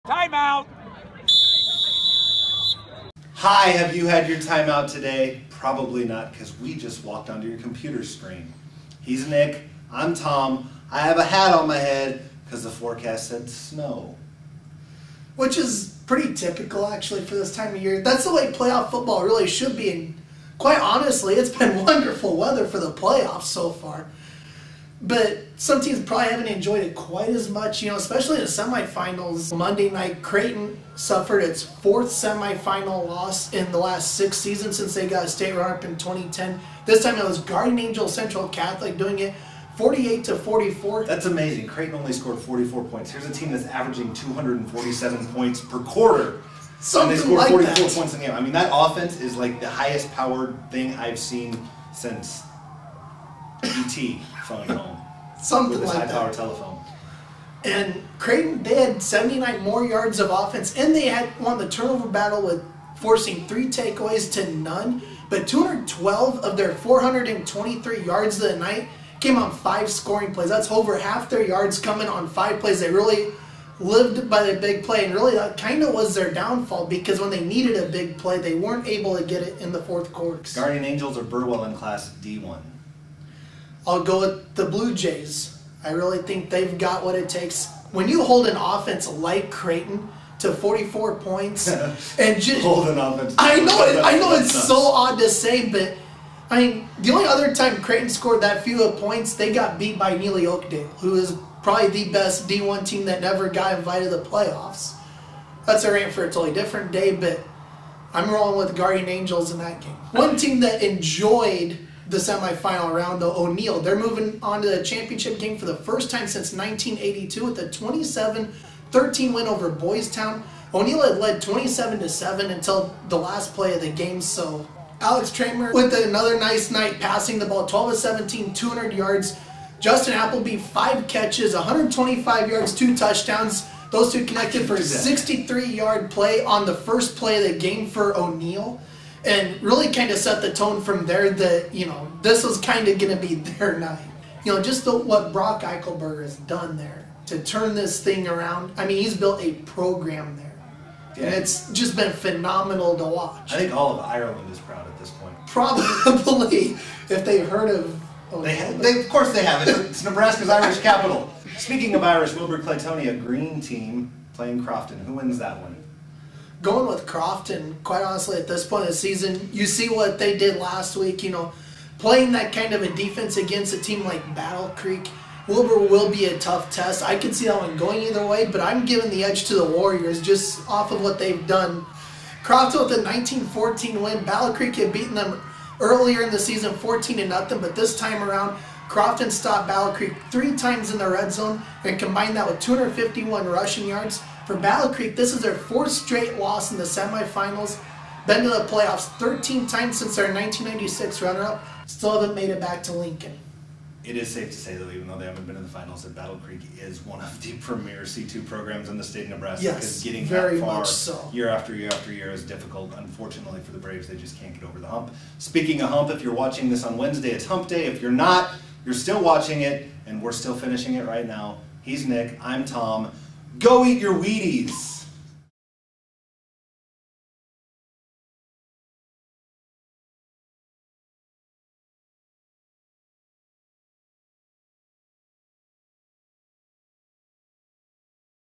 Time Out: GACC, Crofton, O'Neill set to compete for titles
Which teams will return with state titles next week? Sports reporters Nick Benes and Tom Behmer take a look at the semifinal rounds of high school football and discuss the chances O'Neill, Guardian Angels Central Catholic and Crofton have to win it all in the upcoming championship games in this week's episode of Time Out.